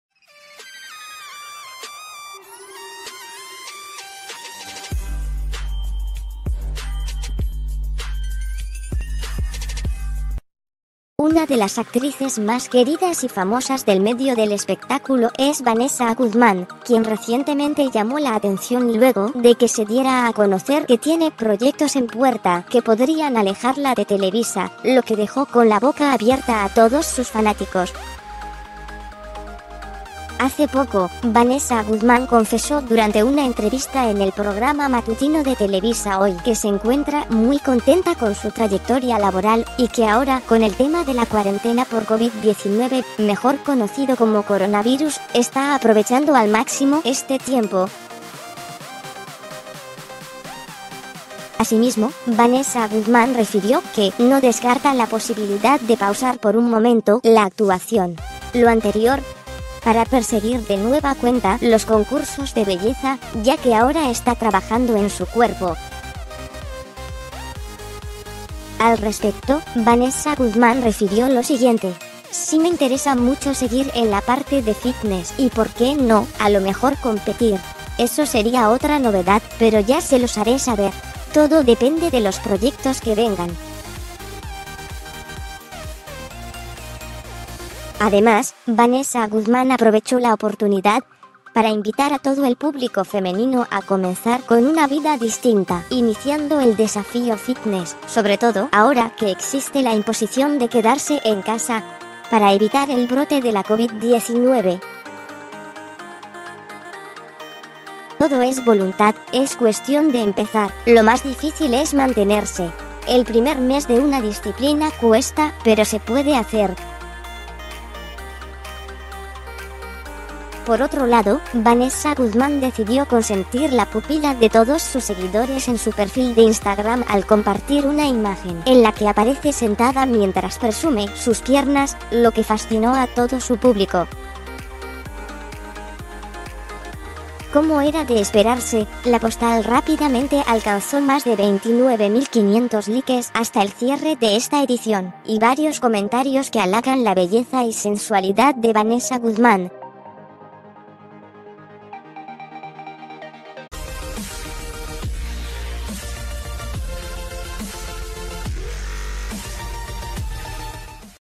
Vanessa Guzmán confiesa interés por proyecto que la alejaría de Televisa
Una de las actrices más queridas y famosas del medio del espectáculo es Vanessa Guzmán, quien recientemente llamó la atención luego de que se diera a conocer que tiene proyectos en puerta que podrían alejarla de Televisa, lo que dejó con la boca abierta a todos sus fanáticos.